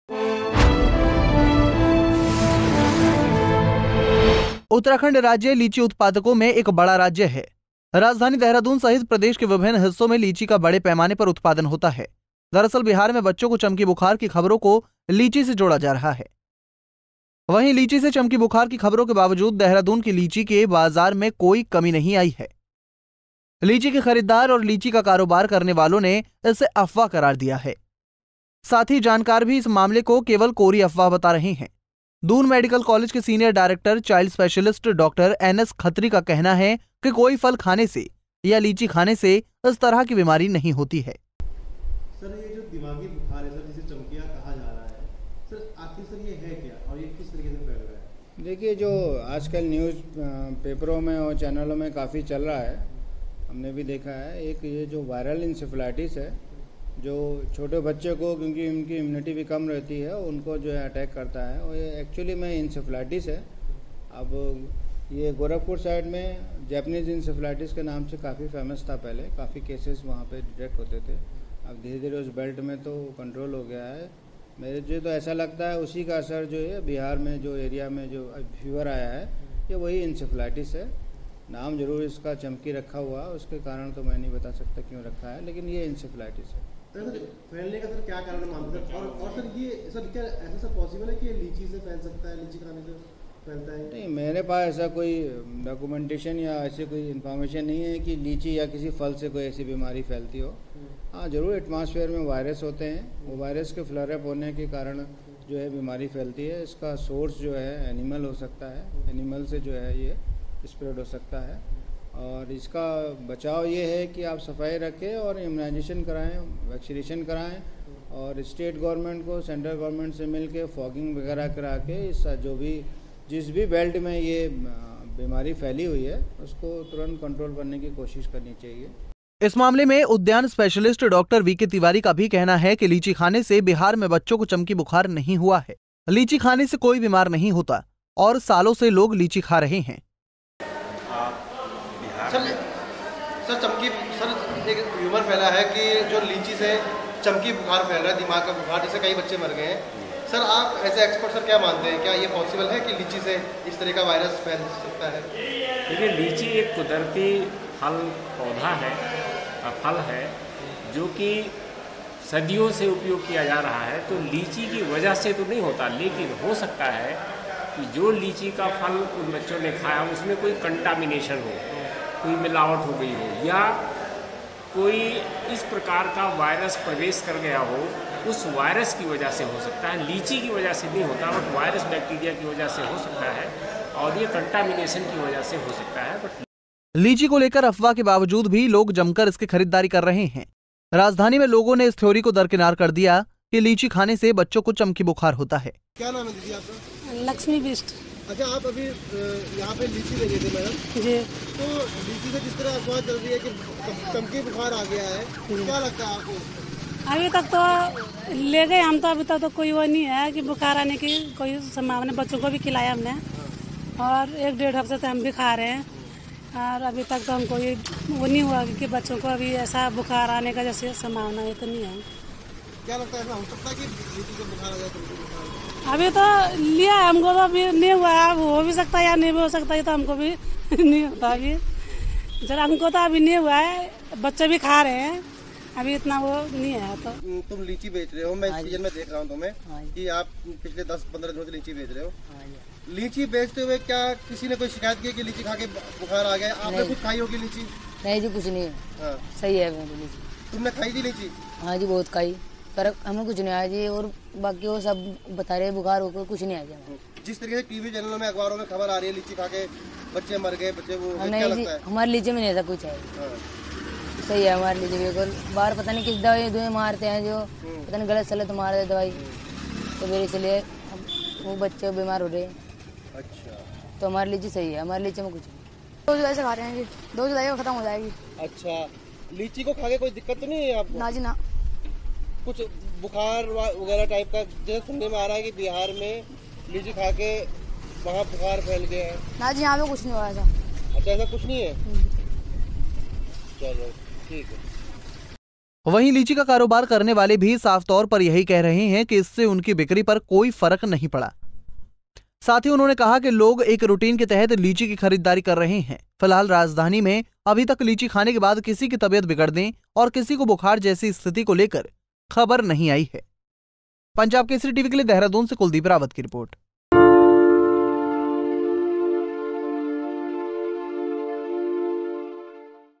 देहरादून में अफवाहों के बाद भी बिक रही हैं लीची
देहरादून में अफवाहों  के बाद भी बिक रही हैं लीची, बिहार में लीची को माना जा रहा है चमकी बुखार का कारण